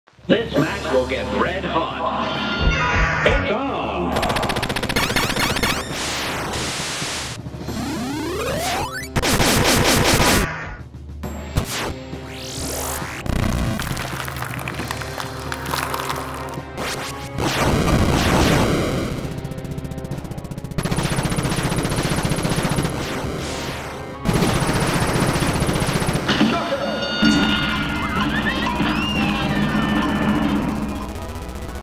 [Sprite Animation] - Cuphead vs Ridley
Cuphead NES Sprites by MisterMike